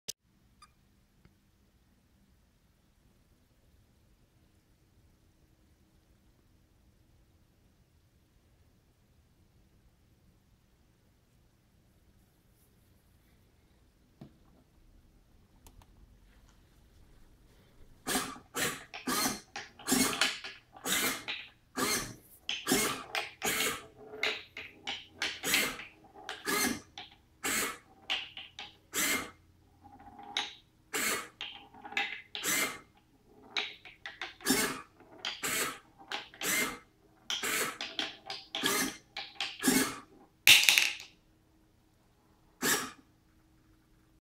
Arduino Robotic Labyrinth
This is a tilting robotic labyrinth that I made for my introductory mechanical engineering course at the University of Minnesota, College of Science and Engineering. The goal of this robot to get a rolling object from start to finish.

Two servomotors attached to adjacent sides of the chassis are used to control tilt. Each one is fastened to a slider crank mechanism fabricated from a wooden stir stick. The tilting plane is constrained in the center by a "Bionicles" universal shoulder joint, and separated from the end of each slider crank by two back-to-back shoulder joints to allow for freedom of motion upon tilting. Since "Bionicles" pieces are made of ABS plastic, zip ties, press-fits, and (expanding and hardening) gorilla glue were used to hold parts together.

The servos are hooked up in parallel to a 6V power source (4 AA batteries) and attached to independent PWM channels on the Arduino. A DIP button switch is used to start the maze and a flashing indicator LED turns off while the program is running.

Wood used is mostly 3/16" MDF board.  The exception is a 10" 2 x 4 which was cut with a band saw and used for supports.